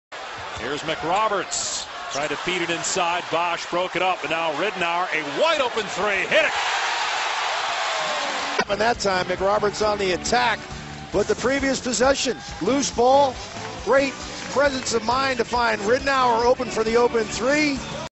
NBA, playoff 2014, Heat vs. Bobcats, Round 1, Game 3, Move 19, Josh McRoberts, assist
Miami Heat - Charlotte Bobcats, Game 3, 26/04/2014, quarter 2
Josh McRoberts, assist